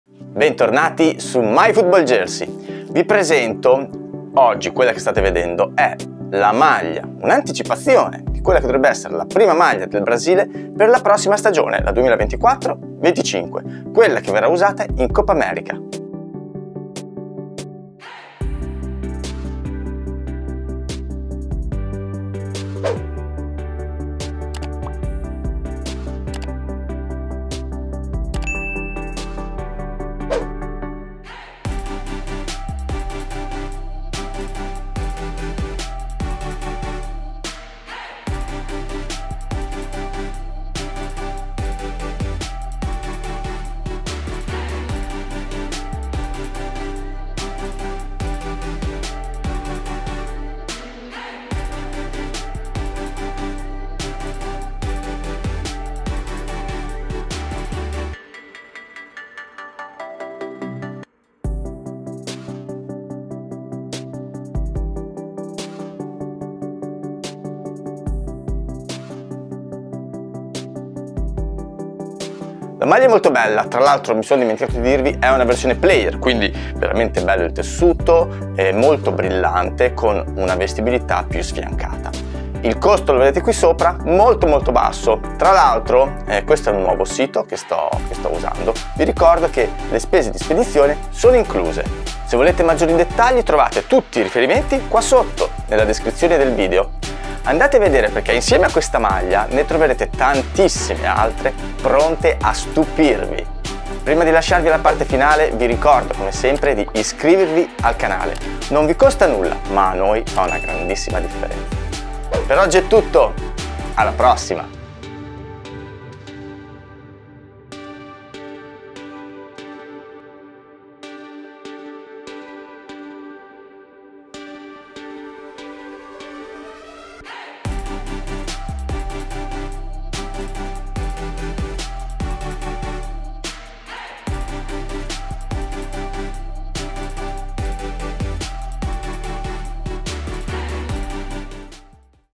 Brazil Home Jersey 24/25 Leaked (Gojersey) Player Version Unboxing Review
Discover the Brazil Home Jersey 24/25 Leaked (Player Version)
Watch till the end to see how the Brazil Home Jersey 24/25 Leaked (Player Version) fits!
If for some reason the link for the Brazil Home Jersey 24/25 doesn't work, write a comment about it, I will reply as soon as possible.

The Brazil Home Jersey 24/25 Leaked (Player Version) costs only 31.95 €! ** Shipping INCLUDED **

Find out the measurements of the Brazil Home Jersey 24/25 Leaked (Player Version).
Size: Large
Height: 175 cm (5 ft 9 in)
Weight: 70 Kg (154 lbs)

Timecodes
00:00 Overview
00:23 Details View
01:12 Pricing
01:55 Dimensions & Try On

Please watch the Brazil Home Jersey 24/25 Leaked (Player Version) video review in 1080p (HD)

Remember this is just a YouTube channel, we don't work for Gojersey or any other website.